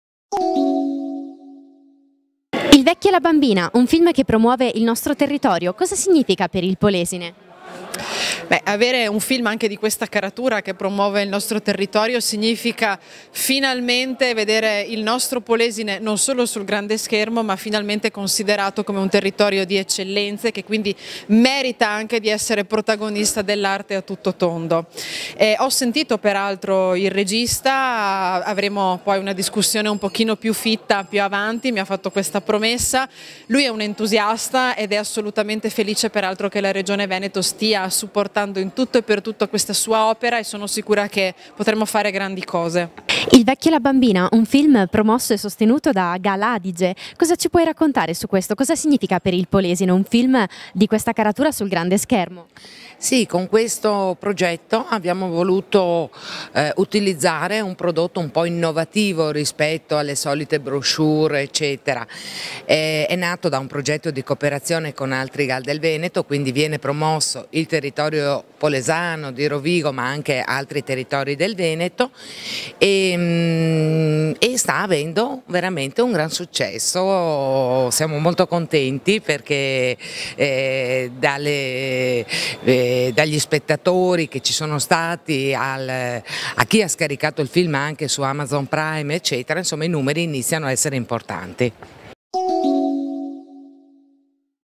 Turismo lento un'opportunità per il Polesine
Intervista a Claudia Rizzi direttrice del GAL Adige